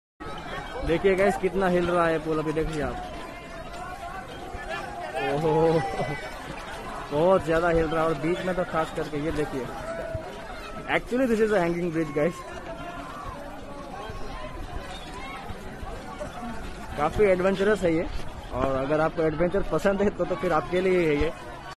gujarat ke morbi pol ka pahila video. मोरबी में पोल ​​हडसा में कितने की जाने जाने morbi
मोरबी में पोल ​​हडसा में कितने की जाने जाने morbi me pol hadsa me kitne ki gae jan